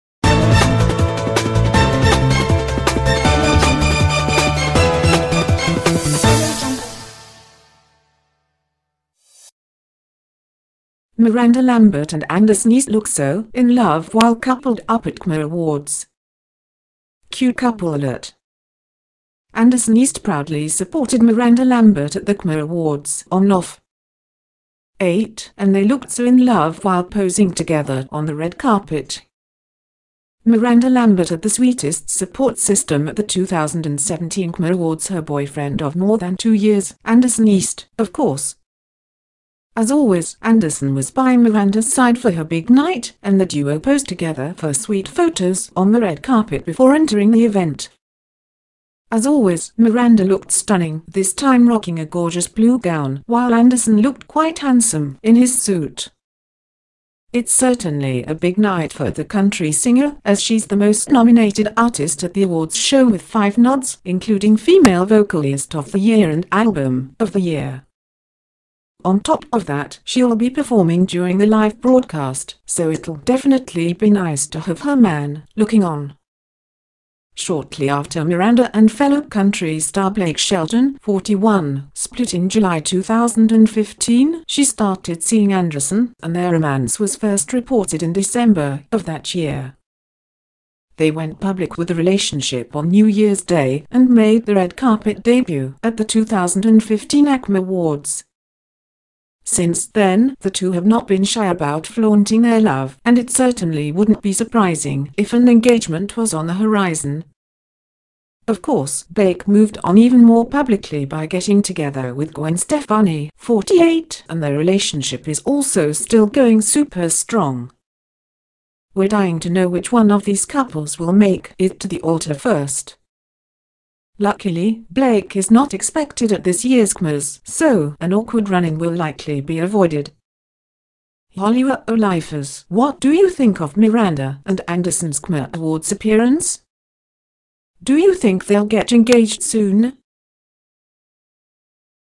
Miranda Lambert & Anderson East Look So In Love While Coupled Up At CMA Awards
Cute couple alert! Anderson East proudly supported Miranda Lambert at the CMA Awards on Nov. 8, and they looked SO in love while posing together on the red carpet.